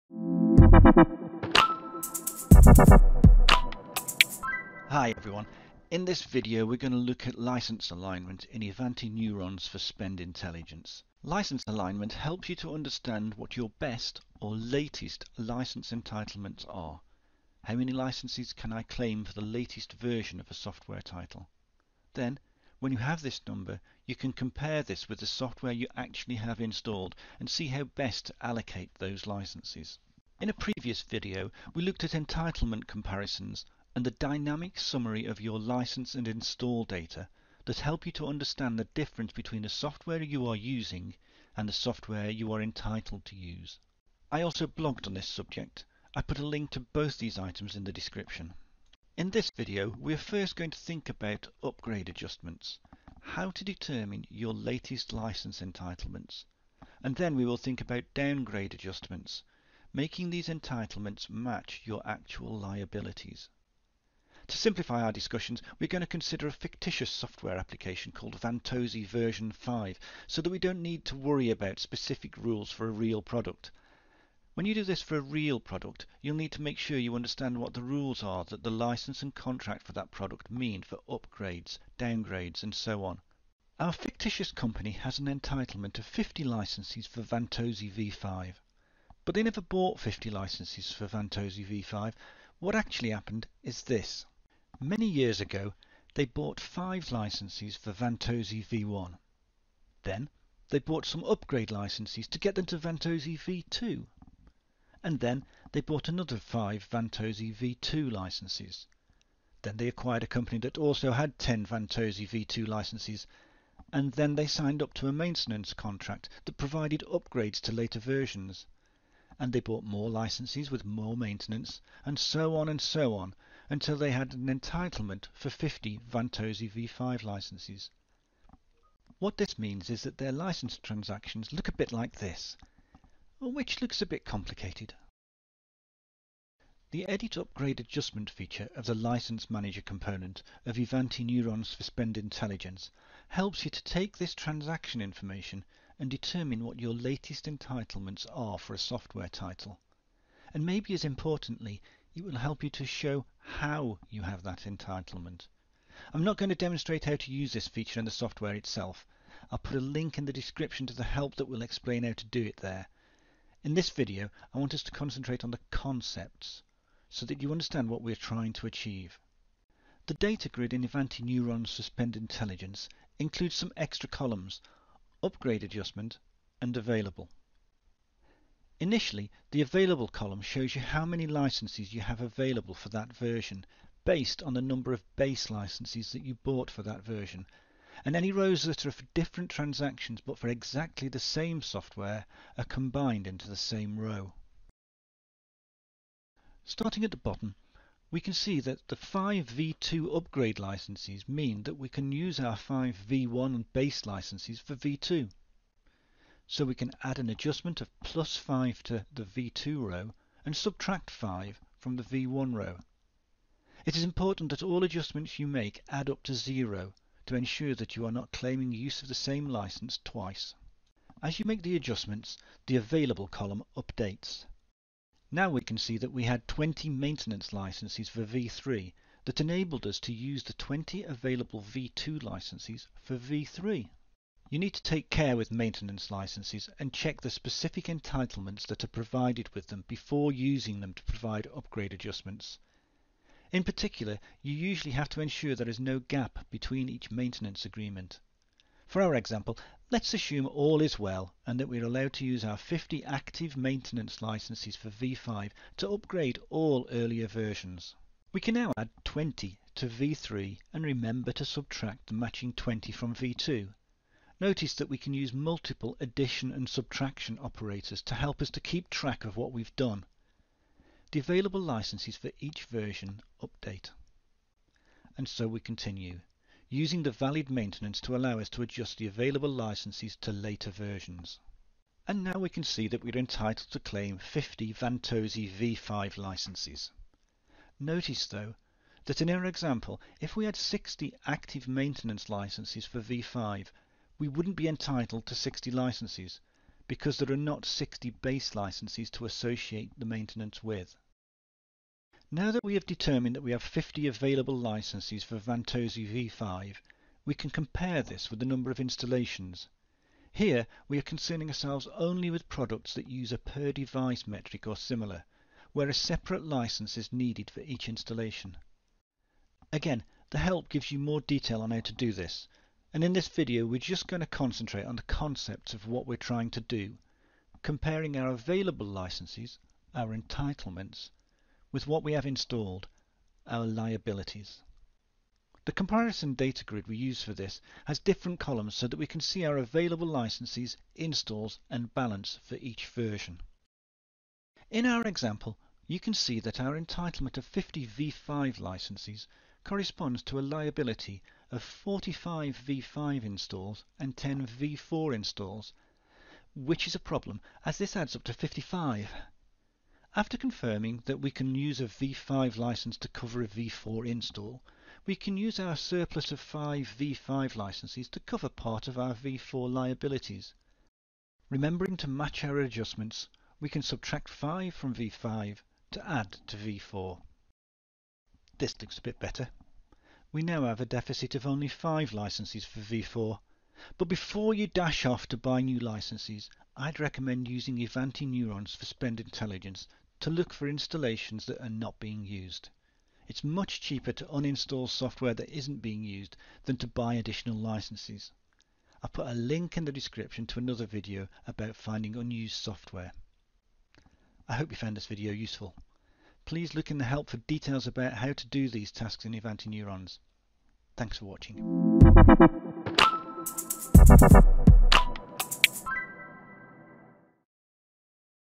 Determining if you have enough licenses for your estate using Ivanti Neurons for Spend Intelligence
License alignment in Ivanti Neurons for Spend Intelligence helps you to understand what your best – or latest – license entitlements are: how many licenses can I claim for the latest version of a software title? Then, when you have this number, you can compare this with the software you actually have installed and see how best to allocate these licenses.

**Remember that if you have installed a piece of software, then you needed to have a license for it. Also, always check the vendor-specific license rules to understand what conditions need to be met for license reassignment**